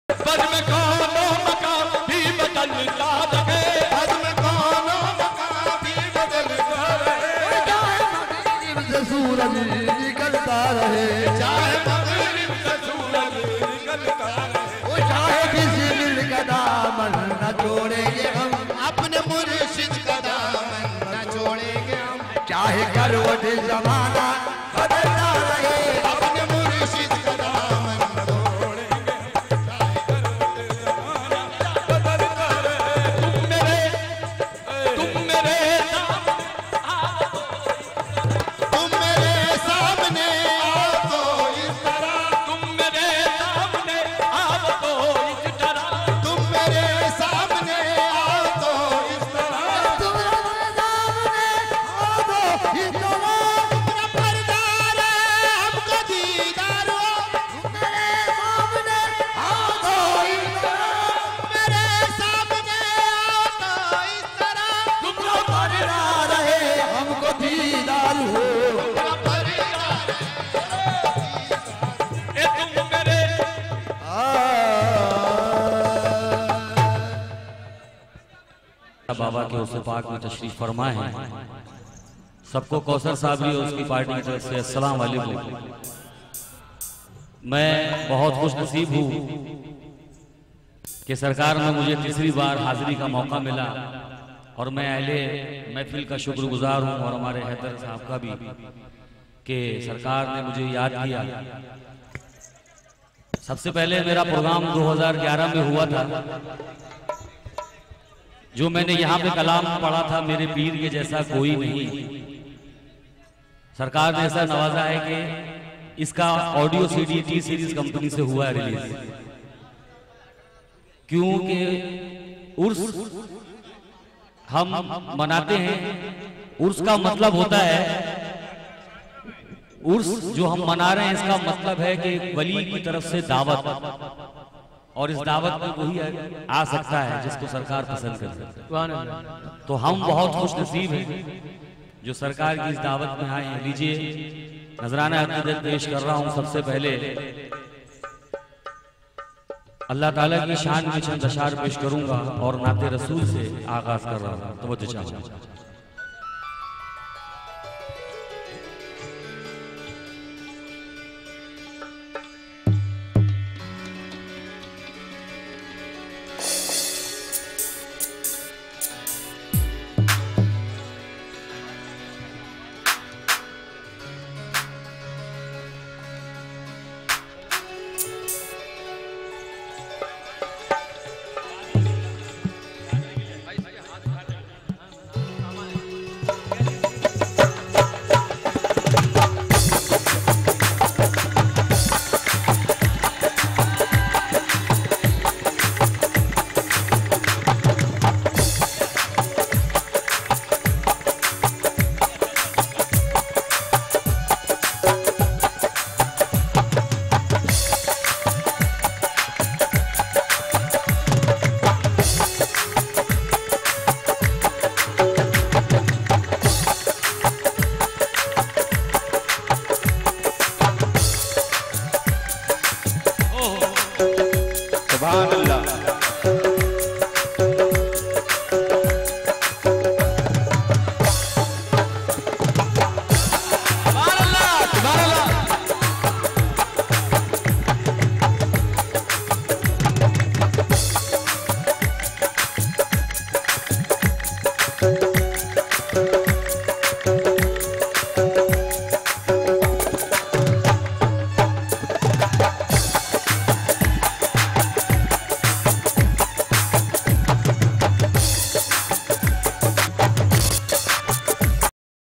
Bismillah Shah 10 th Urs 2013 Mahfile Sama Part 21
Dargah Wa Khanqah Jahangiri Asvi Bismillahi Jari Mari Kurla Andheri Road Mumbai